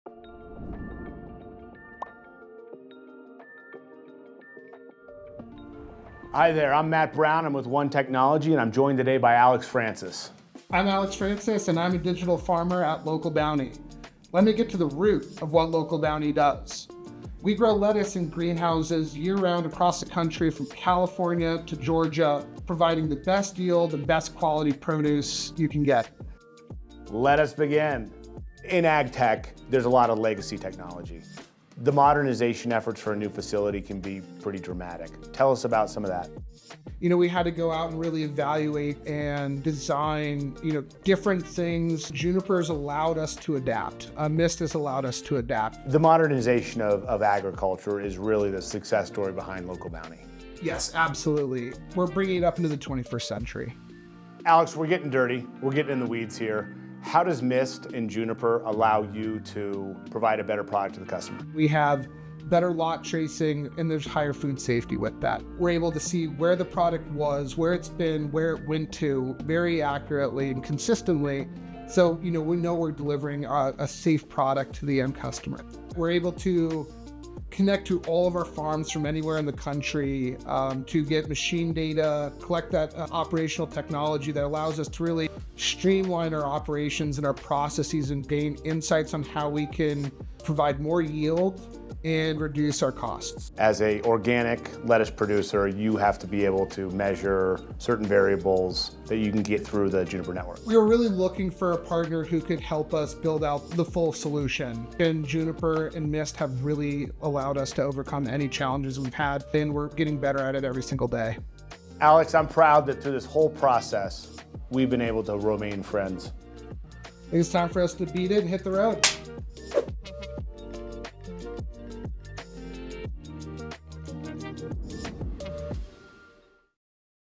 Juniper Mist Video Testimonial with Local Bounti
Alex Francis joins Mat Brown to explain how Local Bounti is leveraging Juniper Networks and Juniper Mist to improve how lettuce is grown, harvested and delivered to their customers!